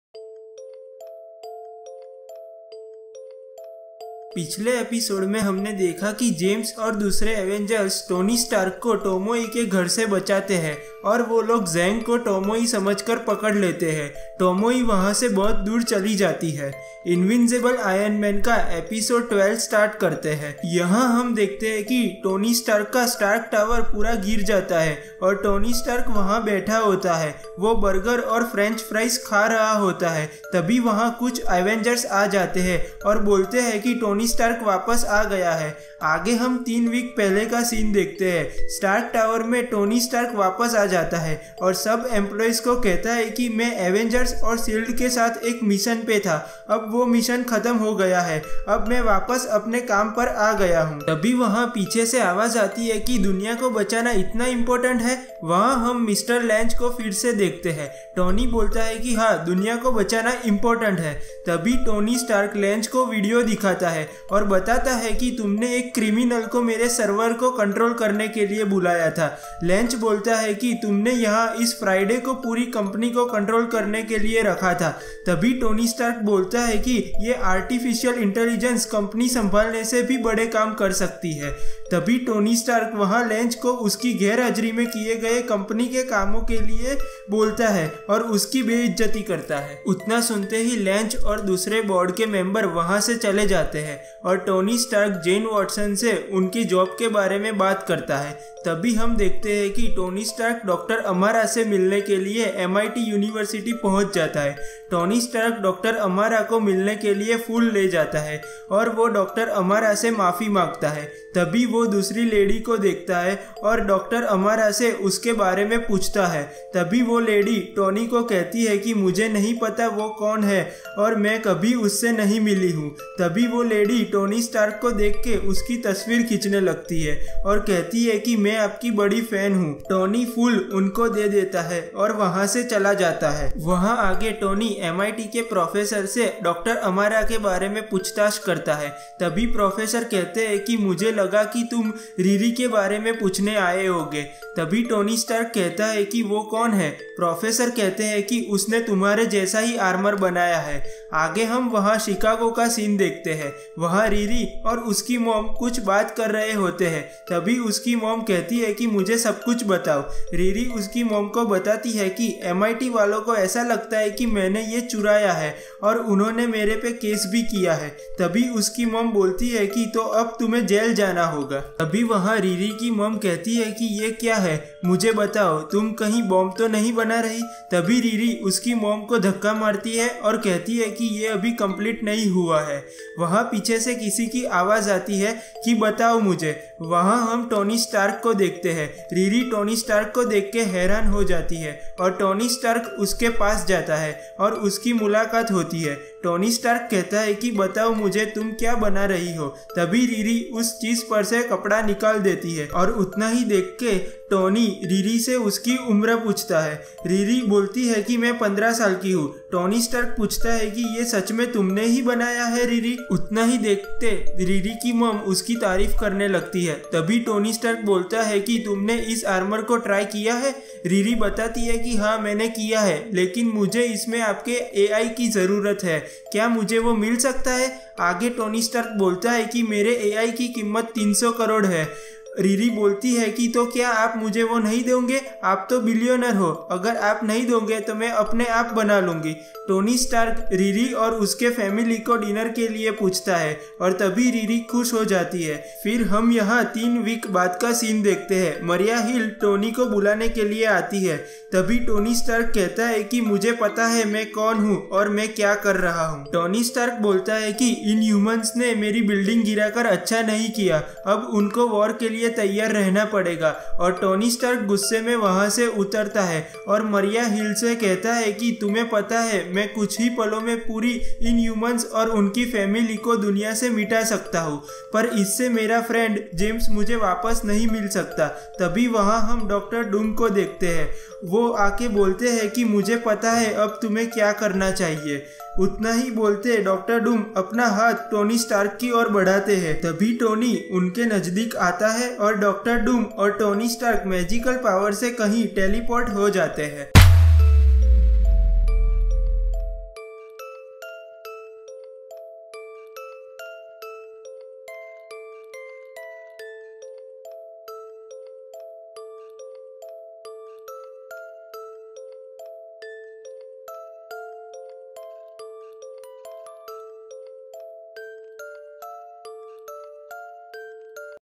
Invincible Iron-Man Ep-12 | Comic Book Story In Hindi | All comic Present
Invincible Iron Man Episode 11 | Comic Book Story In Hindi | Marvel Comic book

Publisher  : Marvel Comics
 Total Issues :  14
 Dates Published  : 2015, December — 2016, December
 Creators  :  Brian Michael Bendis David Marquez
 Featured : Iron Man
 Crossovers : Civil War II

STORY :
The members of Stark Industries' board of directors arrive to Stark Tower, accompanied by the Ghost with the intention to seize the company. Friday attempts at reasoning with them, but Mister Lynch insists the fellow members of the board to ignore her. They arrive at Stark's lab, and send Ghost to break into it to override the secure servers. He phases through the vault door, opens the door, after a few seconds comes back confused. The board enters the lab and finds Mary Jane Watson, claiming to be Stark's new executive administrator. Together with Friday, she convinces them that the company is in safe hands while Tony is allegedly in a retreat working on new breakout tech to help the company rebound from the recent financial issues.

Outside the New Mexico State Penitentiary, two inmates are escaping in a truck when the armor-clad Riri Williams lands in front of them, with the intention to stop them. The inmates try to run her over, but she successfully causes the truck to flip over with a punch, though at the cost of her armor's integrity. Several New Mexico State Police cars arrive to the scene as the truck continues rolling in the dirt. The State Troopers try to apprehend her, but she clarifies that she's one of the good guys, though it was her first "super hero thing."

On the other side of the globe, War Machine arrives to the Kumamoto North Police Station. Meanwhile, in Osaka, Japan, his arrival catches Tomoe's attention, who is informed of Rhodes' return by Zhang; however, Zhang informs her that other heroes like Thor have been spotted. Tomoe scolds herself for allowing her organization to lose anonymity. They try to consider all of their options, including going into hiding, but Tomoe decides to attack the heroes before they could surprise her.

Under his disguise of Richard Franco, Tony Stark walks down the streets of Osaka when he notices the Vision flying by. He enters a coffee shop, locks himself in the bathroom, and notices Rhodes sitting in the lavatory after turning his back. Tony questions Rhodey's presence in their emergency meet place, he replies he's there to rescue Tony, but Stark denies to need help. When Rhodey tries to convince Tony to deactivate his facial disguise, Stark demands him to drop the cover first. The person in front of Tony then changes her appearance from Rhodey to her original look, that of Ms. Marvel. When Stark questions Ms. Marvel's presence, she gives her a message from Rhodey, that he won't be sorry for what he's about to do. She also talks to somebody on her phone and informs them that she has Stark.

Tomoe's base of operations is suddenly attacked by the Avengers and S.H.I.E.L.D. forces. Ms. Marvel brings Tony once the battle has already been won, and numerous biohack ninja are being arrested. War Machine says "You're welcome." to Stark, even though Tony claims that he didn't need help, and still has yet to figure out the entirety of Tomoe's operations. When Rhodes shows Tony that they had also captured the leader, Tony informs him that they had the wrong person. They had captured Zhang instead of Tomoe.

Zhang is later thrown into a jail cell together with Tony, who uses his identity of Richard Franco to try to get any kind of lead on Tomoe. Zhang informs him that she has no idea of Tomoe's whereabouts, but assures him that Tomoe is much more than she lets on, and when she resurfaces, it will be the end of her enemies.

As the Helicarrier approaches New York City, Tony tells Rhodey that he still doesn't know what Madame Masque stole from him, nor why Tomoe wanted it so badly that she risked her circle's anonymity for it. He also reaffirms that he didn't need help. Rhodey reassures him that he did, stating that Tony isn't a S.H.I.E.L.D. agent or an intelligence agent, and he was using his infiltration as an excuse to escape from his personal problems. Tony eventually admits that it's true, and Rhodey reminds him to go back to his life and inform everyone he's still alive. A Tony communicates with Friday, the A.I. informs him about Riri William's recent actions.